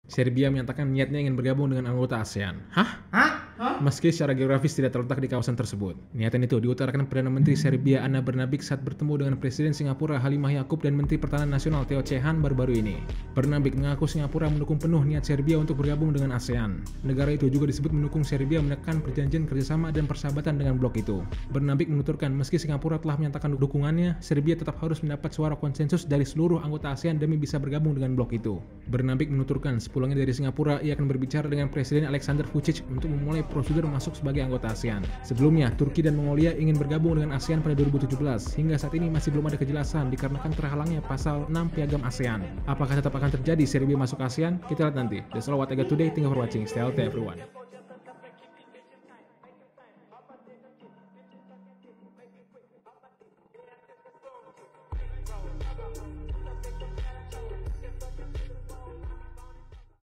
GEGER! SERBIA MAU JOIN ASEAN?
PM Serbia, Ana Brnabic berbicara dengan Presiden Singapura Halimah Yacob ingin bergabung ke ASEAN.
Turki dan Mongolia pada 2017 sudah disupport oleh Filipina untuk masuk ke ASEAN, tapi hingga saat ini masih belum ada kejelasan karena terbendung Pasal 6 Piagam ASEAN yang dimana wilayah yang ingin bergabung harus di Asia Tenggara.